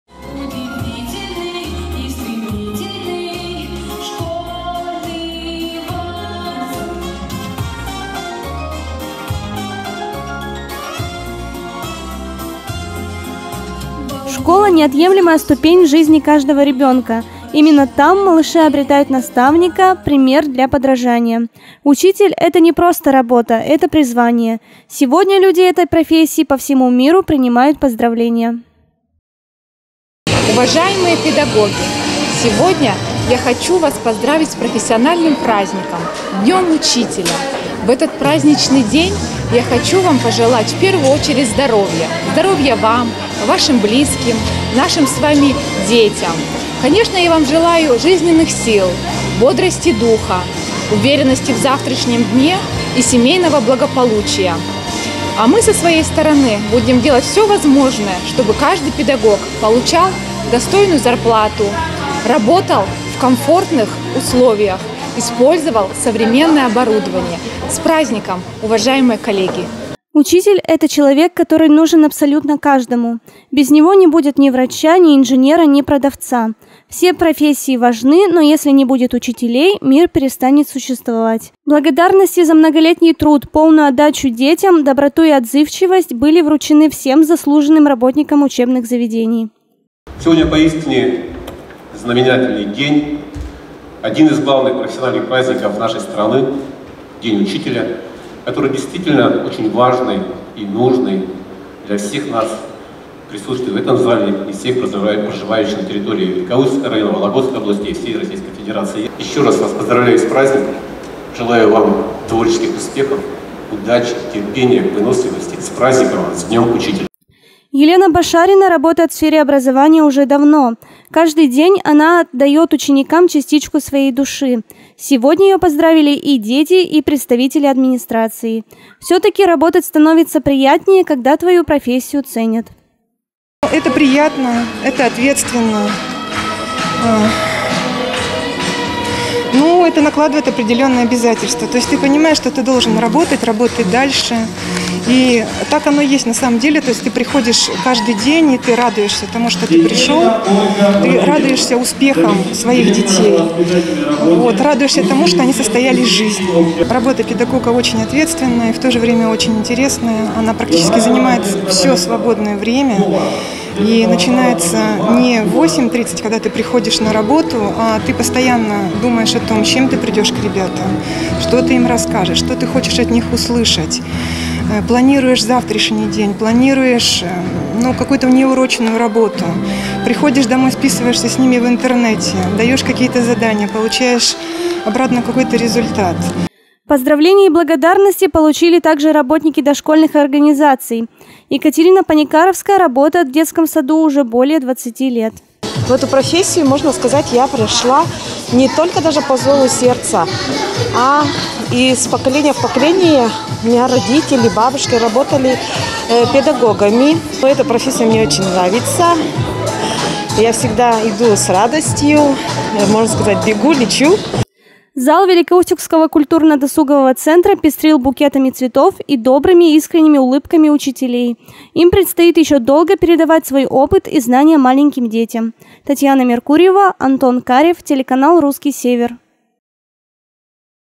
Не просто работа, а призвание
Сегодня профессиональный праздник отмечают учителя